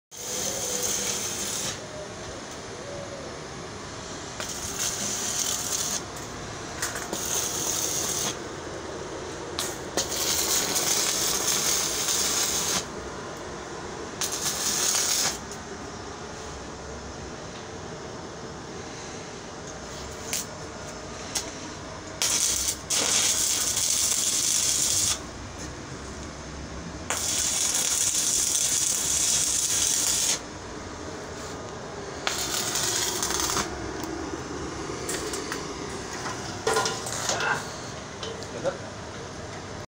Welding the first panel on the blue fence
This is a long fence we built on June 2018. The bit in this video is the first section I welded in place after removing the old wire fence. The fence is right next to the train tracks, so we had to juggle a bit on the edge of the wall to build/install this. It turned out very nice though!